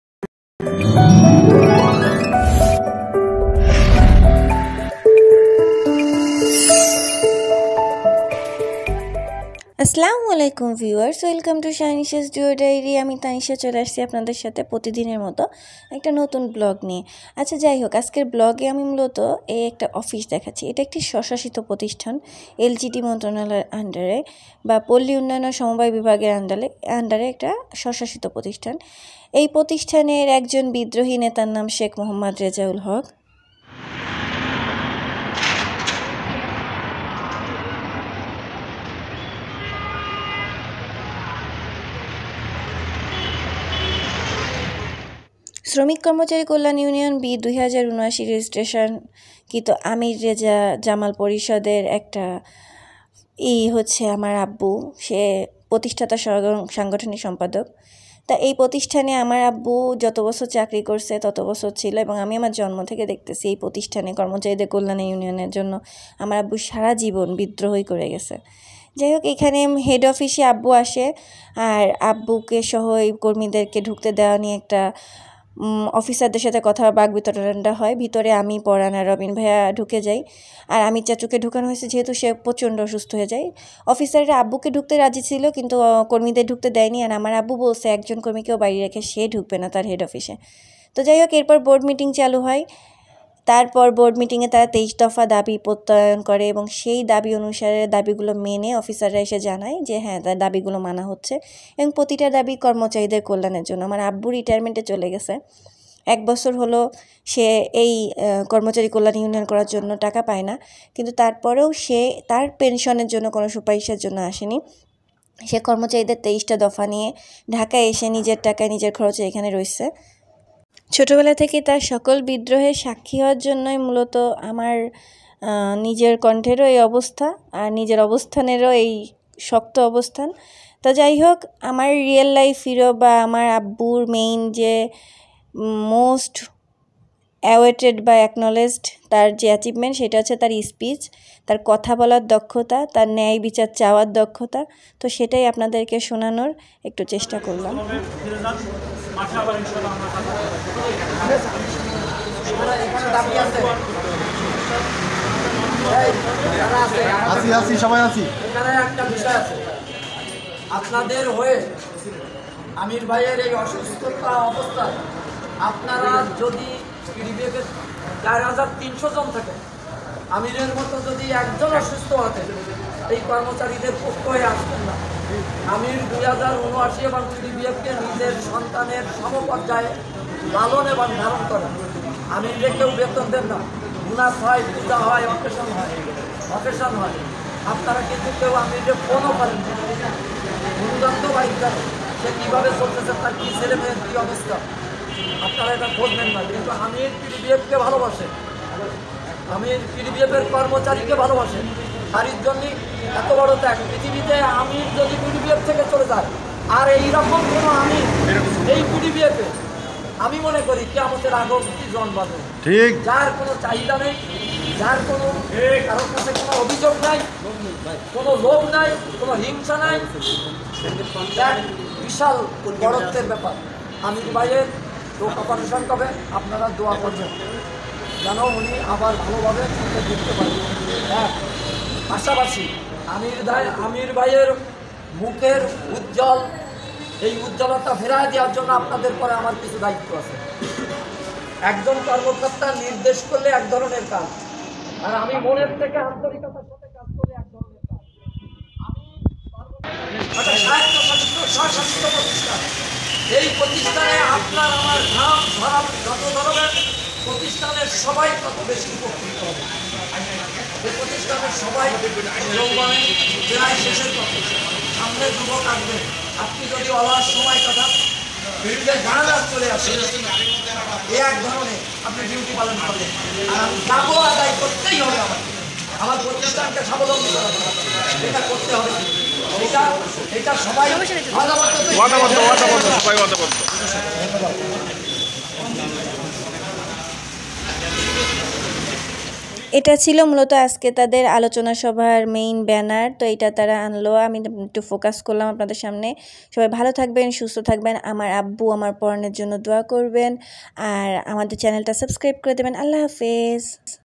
Remembering the Martyrs: A Legacy of Long March #2024
Long March to Life.বৈষম্যবিরোধী  ছাত্র আন্দোলনে শহীদদের রুহের মাগফেরাত কামনায় বি-২০৭৯ স্থানীয় সরকার মন্ত্রনালয়ের অধীনভুক্ত একটি প্রতিষ্ঠান পল্লী দারিদ্র্য বিমোচন ফাউন্ডেশনের কেন্দ্রীয় কার্যালয়ে কর্মচারীবৃন্দ। এই কর্মচারী কল্যান কমিটির প্রধান ব্যক্তি আমার আব্বু শেখ রেজাউল হক। এজন্য ‎@Shahraanishah13 এখানে।